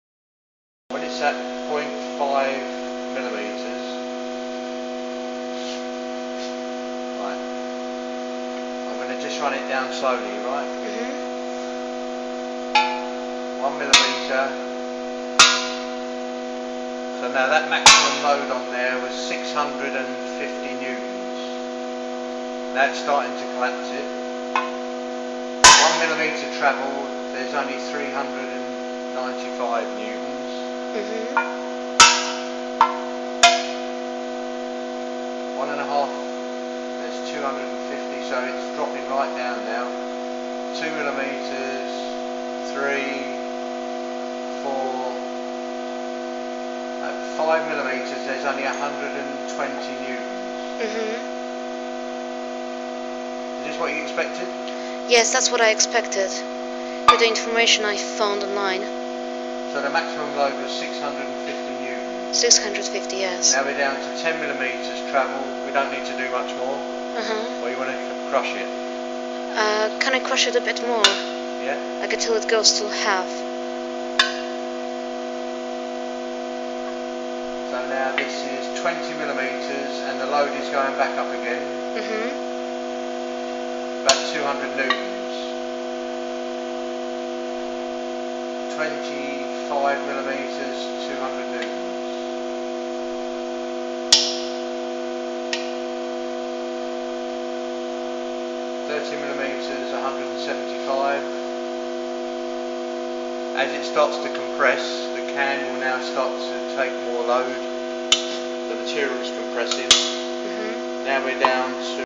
Can crush test with newton meter
A video showing the test we did, crushing a can with a newton meter to measure the force needed to crush a can

Group 4